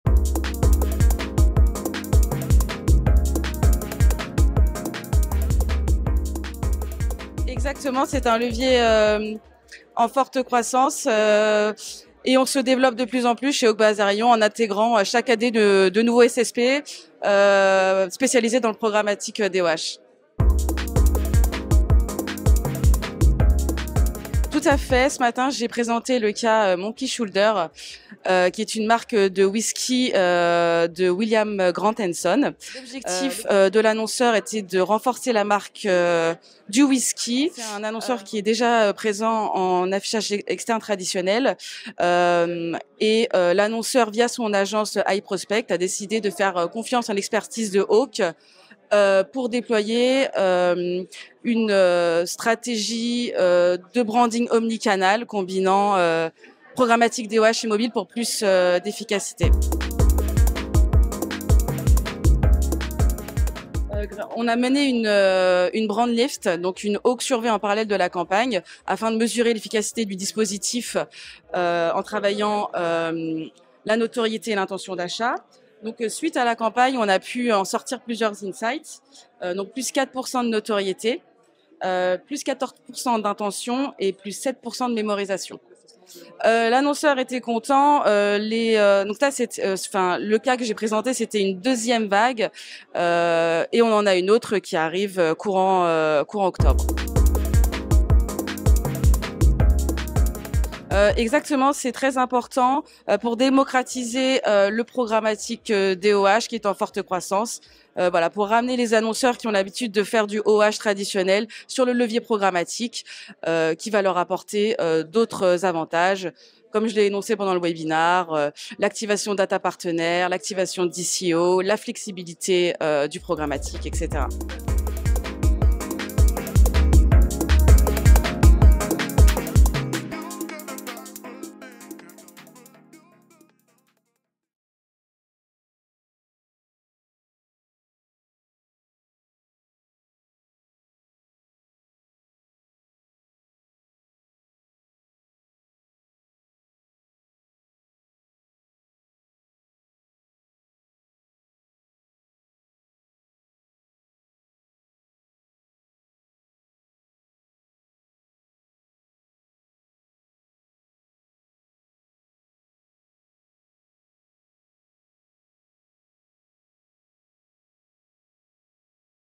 Eva BARDOT, HAWK : "Notre campagne DOOH a augmenté la notoriété de l'annonceur de 4%"
Head of Publisher FR & BE chez Hawk (Groupe Azerion), Eva Bardot était l'invitée d'un Webinar Alliance Digital consacré au pDOOH, le "Digital Out of Home" programmatique.